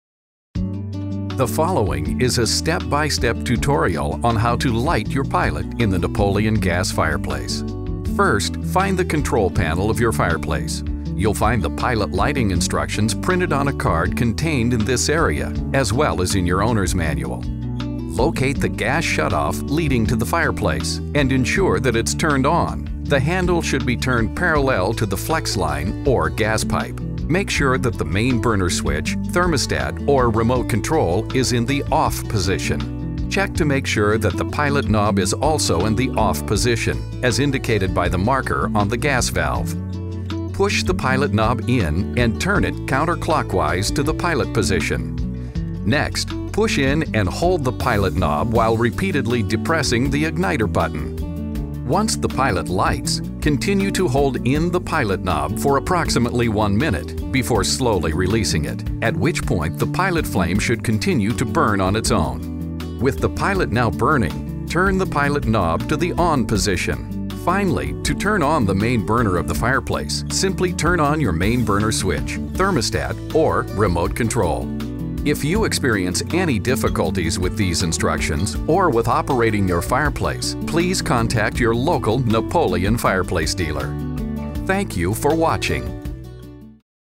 How to light your pilot in a Napoleon Gas Fireplace
Tutorial with step by step instructions on lighting the pilot on your Napoleon Gas Fireplace.